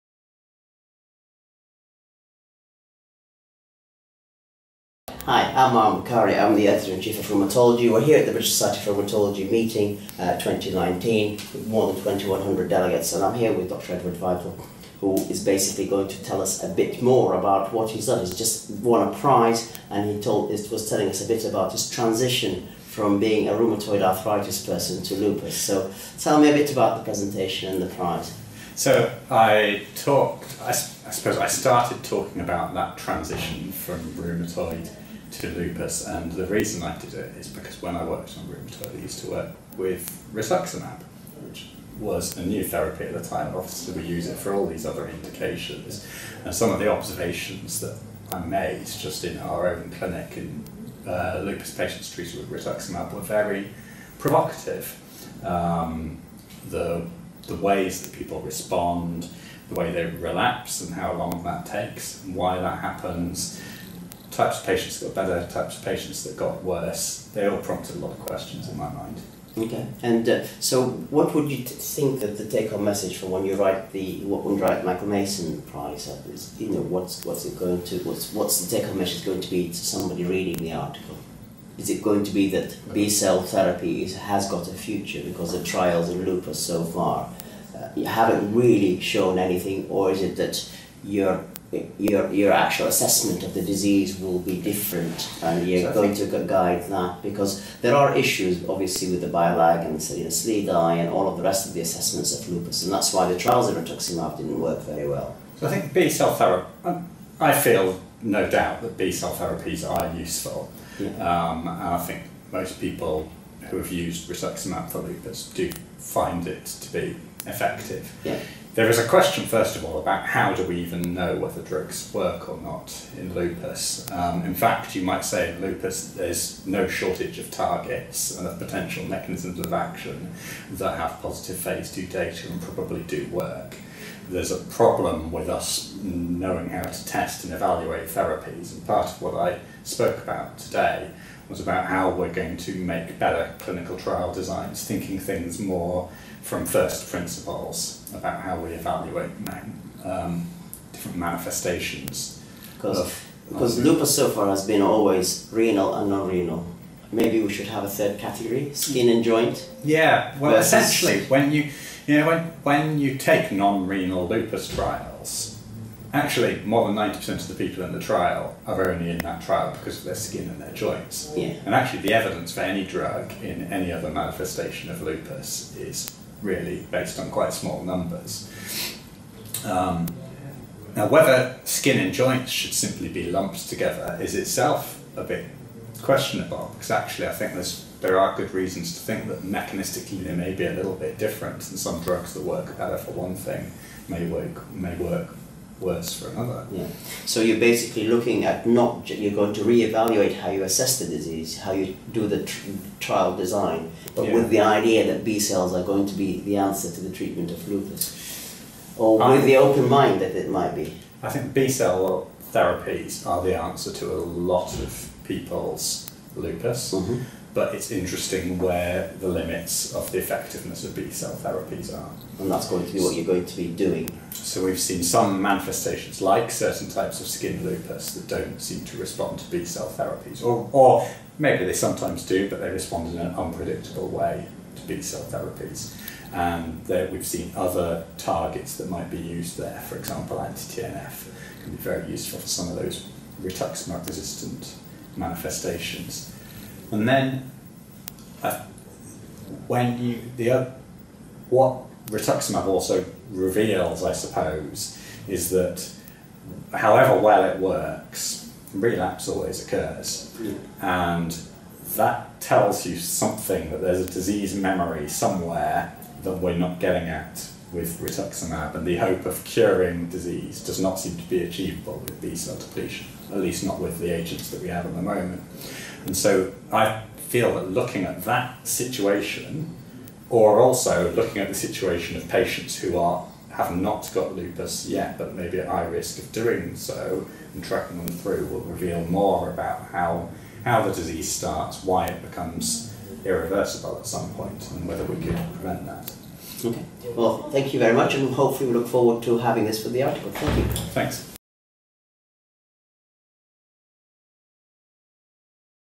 Dr Marwan Bukhari talks to Dr Edward Vital on his Michael Mason prize-winning presentation
Editor in Chief of Rheumatology talks to Dr Edward Vital on his Michael Mason prize-winning presentation at the 2019 BSR Annual Conference in Birmingham, UK.